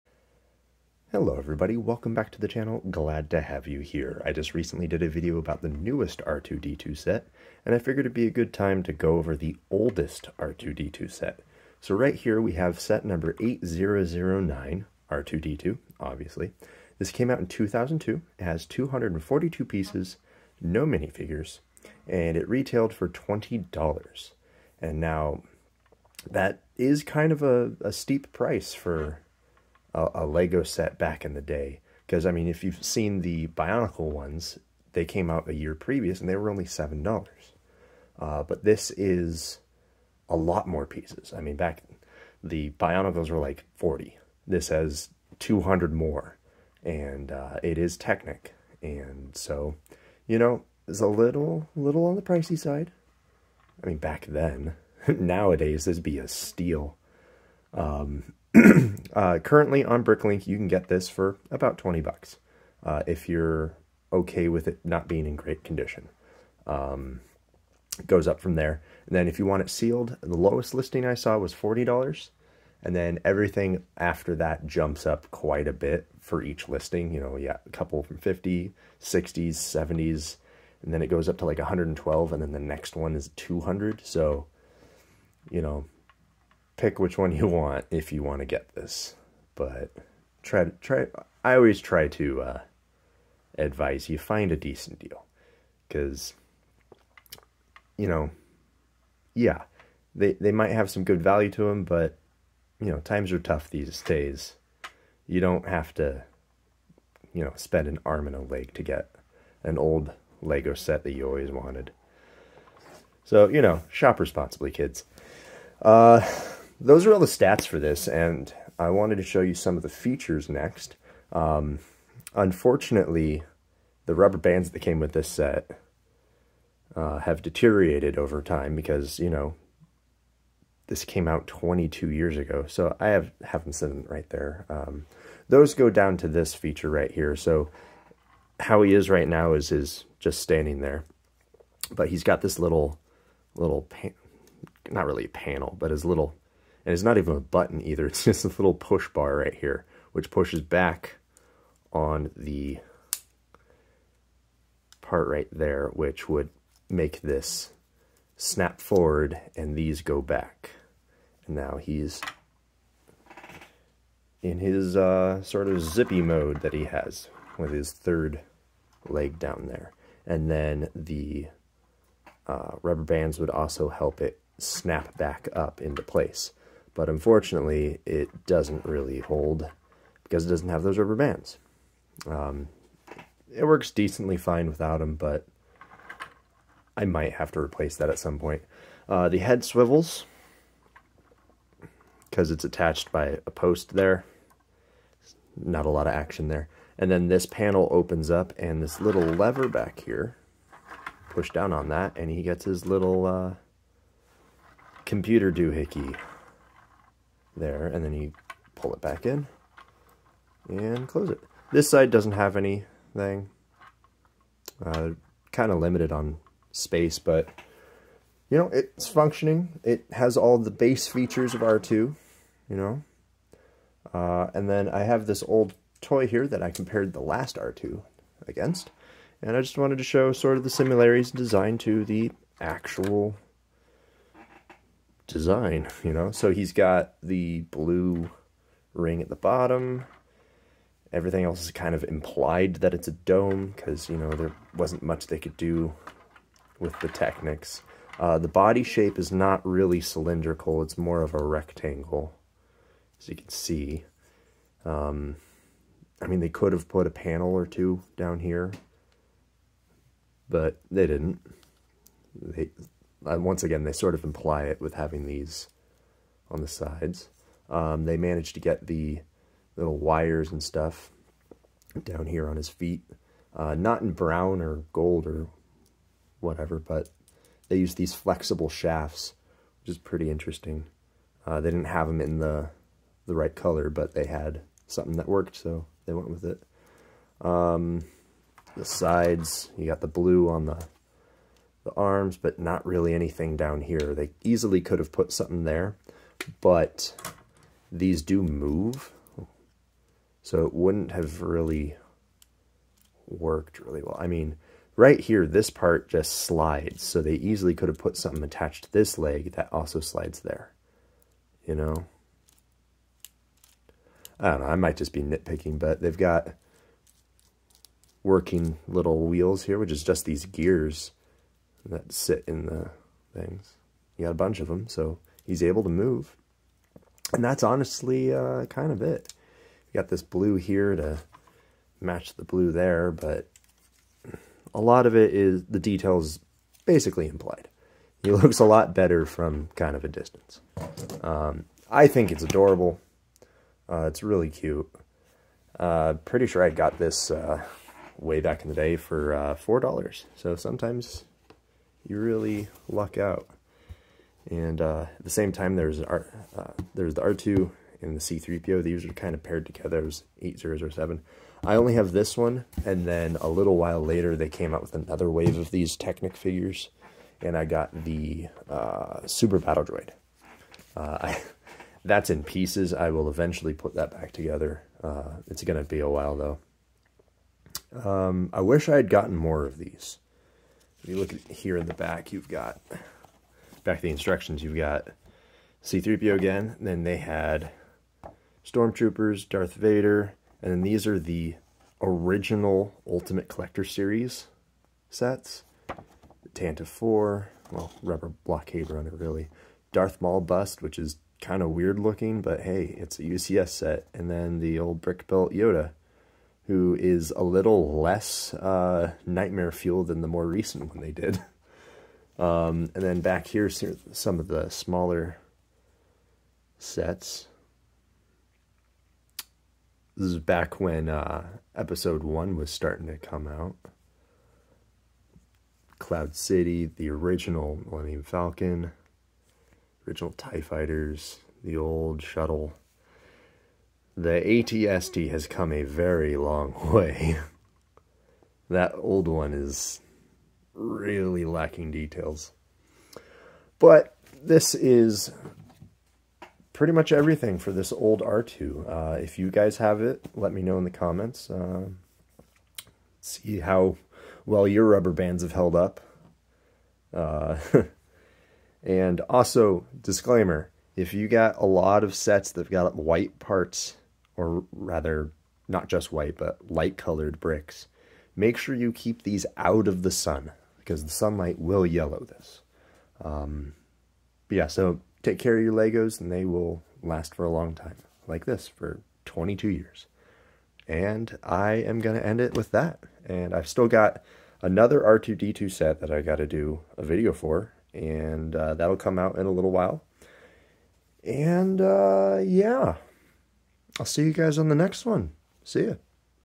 The OG R2 From '02
The very first buildable R2-D2, in all its technic glory